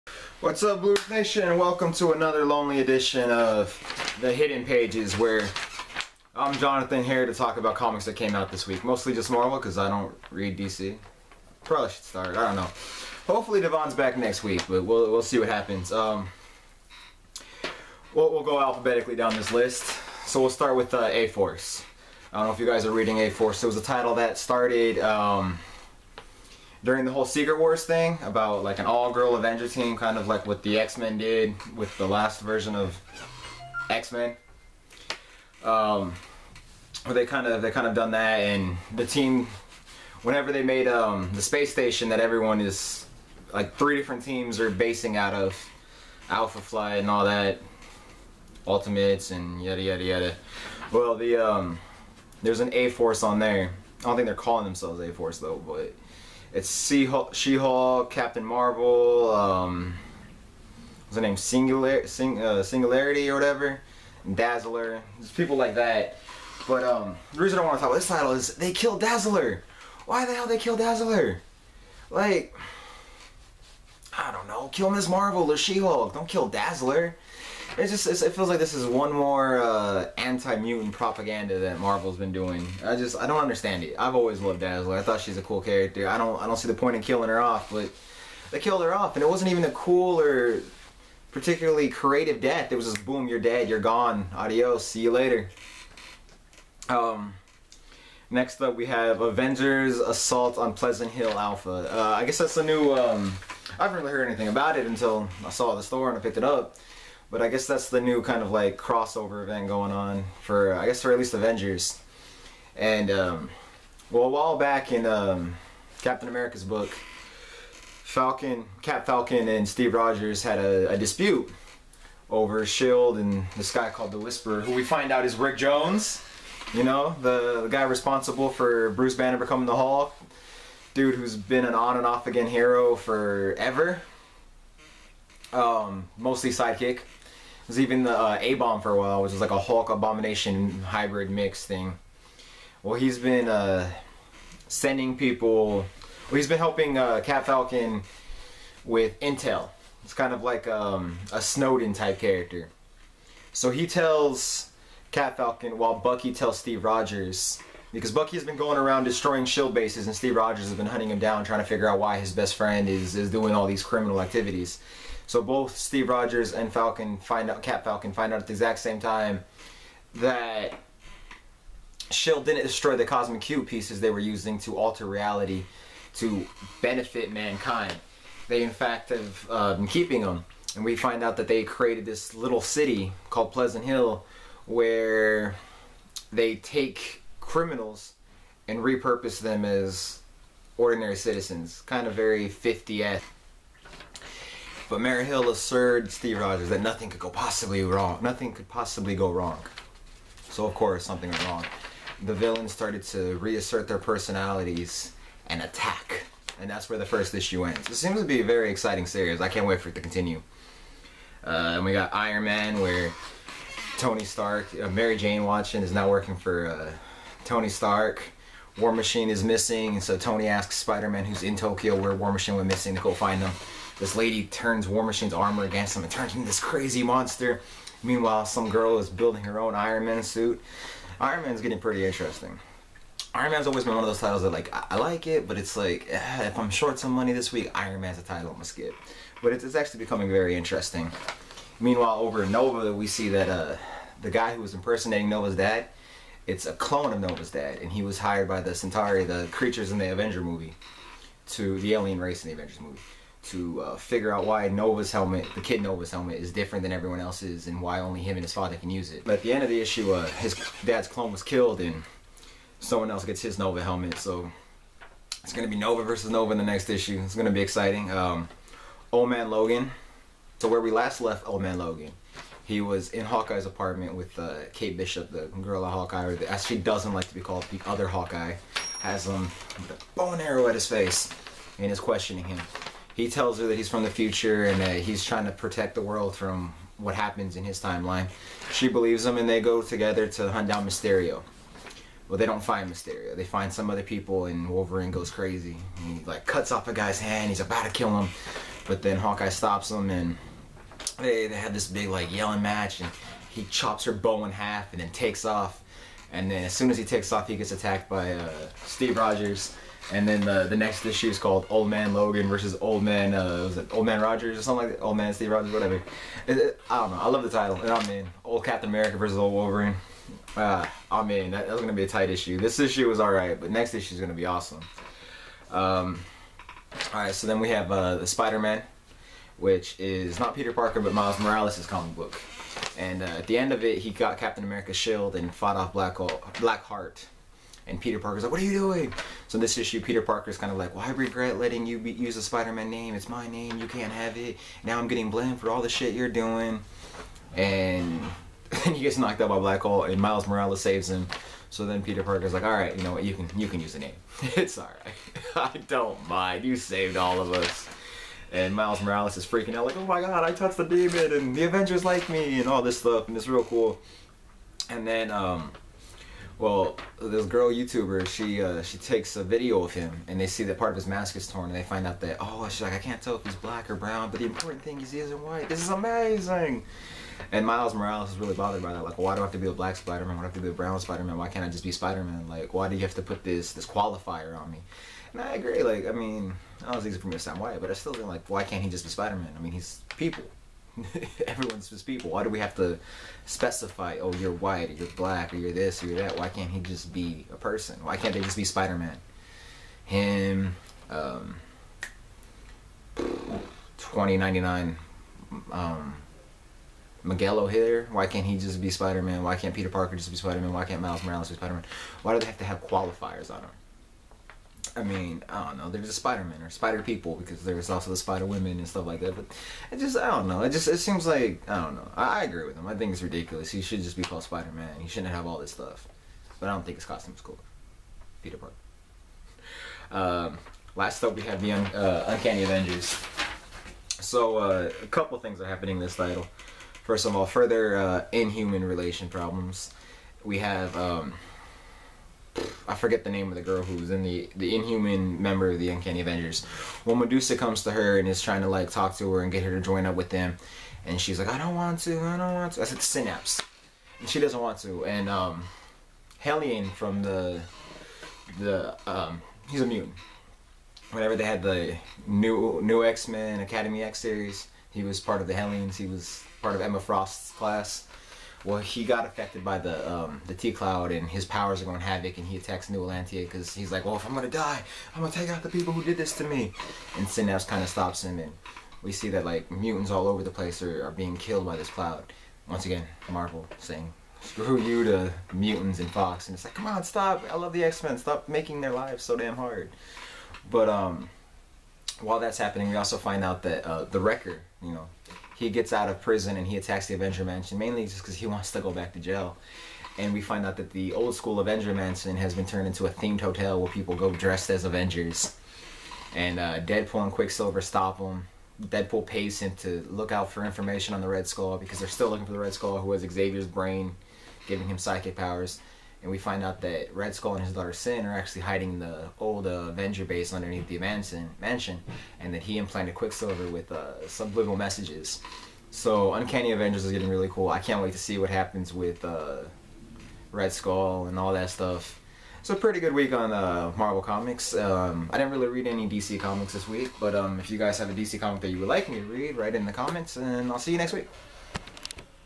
The Hidden Pages #4 Poor Mutants
Once again Jon is alone as he dives into last weeks comic books.

see us all over the web at